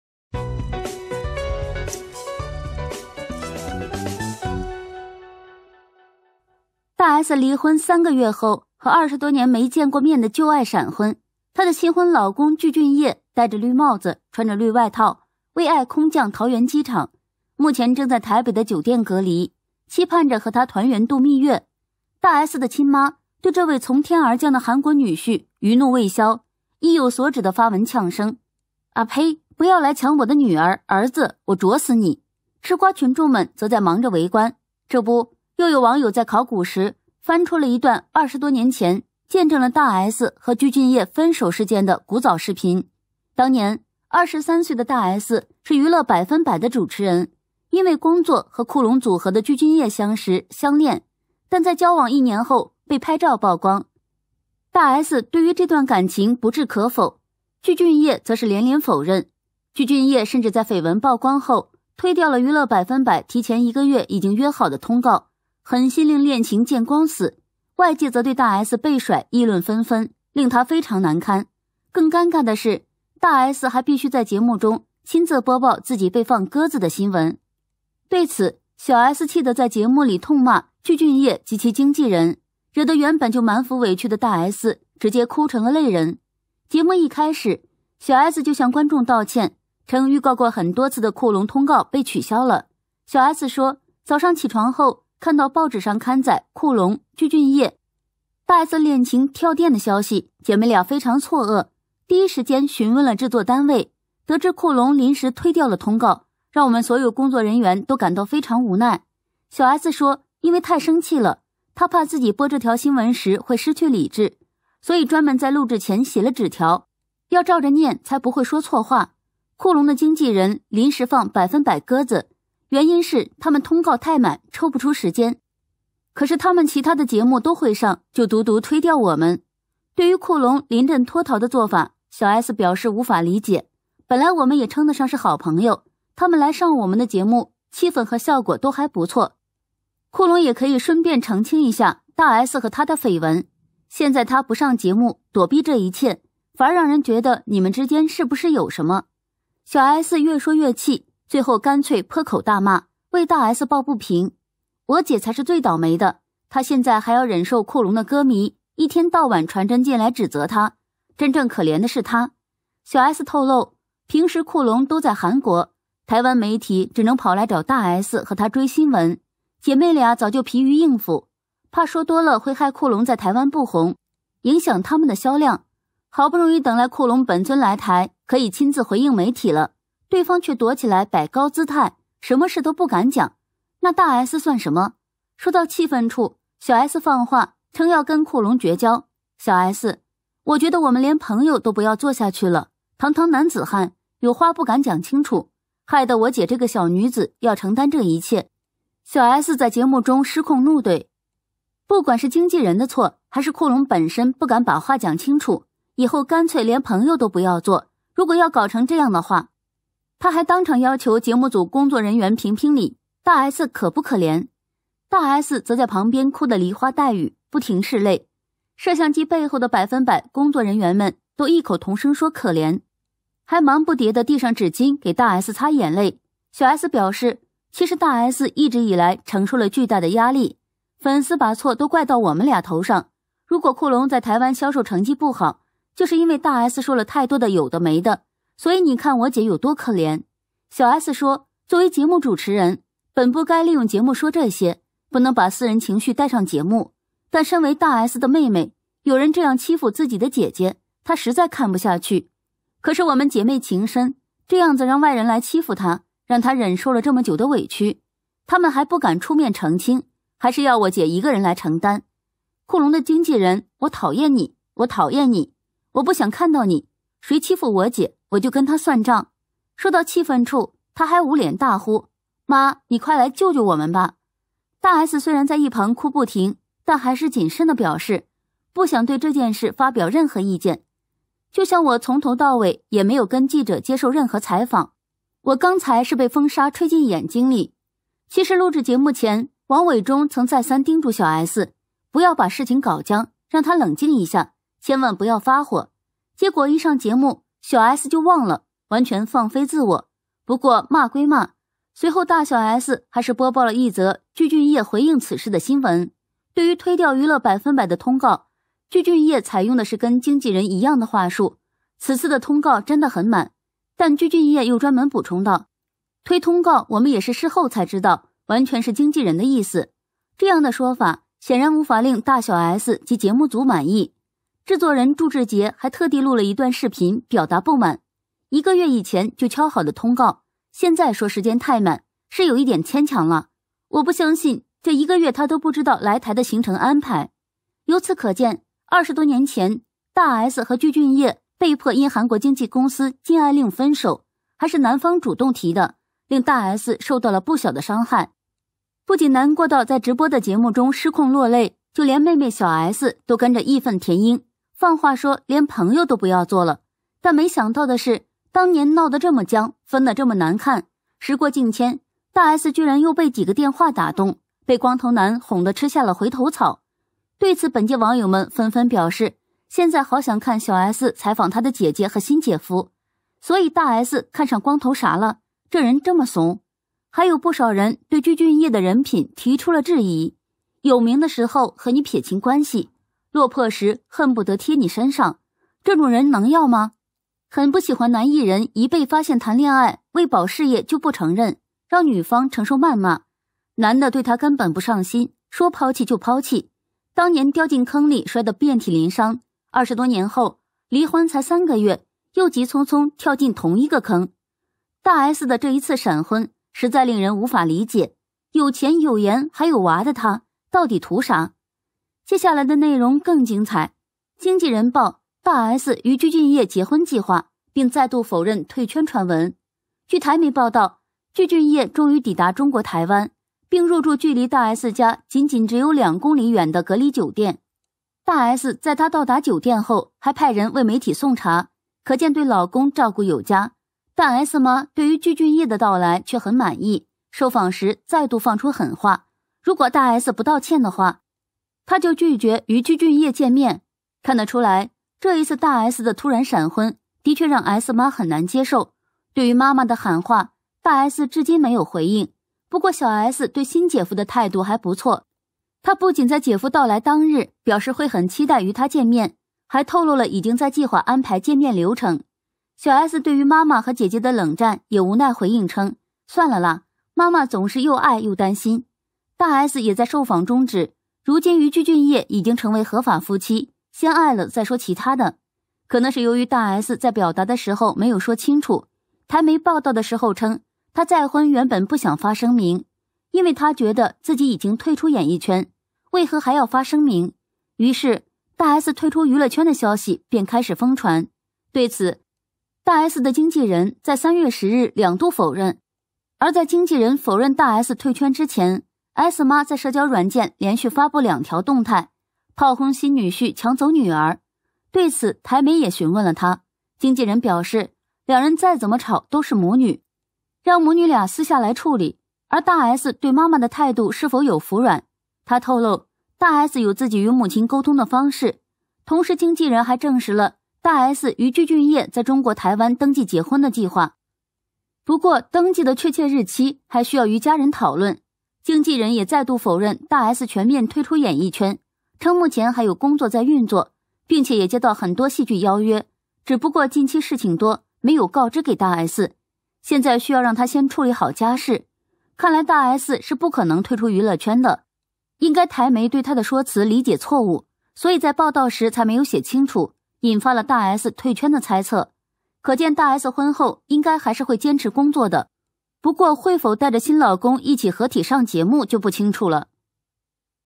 大S爆哭！早年分手視頻曝光，被具俊曄狠甩，小S錄節目破口大罵︱大S徐熙媛︱汪小菲︱小S#娛樂星聞
大S爆哭！早年分手視頻曝光，被具俊曄狠甩，小S錄節目破口大罵